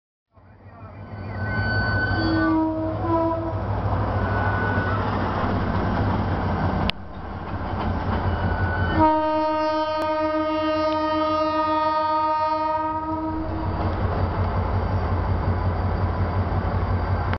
La GT22 A920 hacia Escalada - LGR
video Filmado desde la 9085 de Ferrobaires de la locomotora GT22 A920 yendo a deposito de Remedios de Escalada saliendo de Plaza Constitucion Anden 13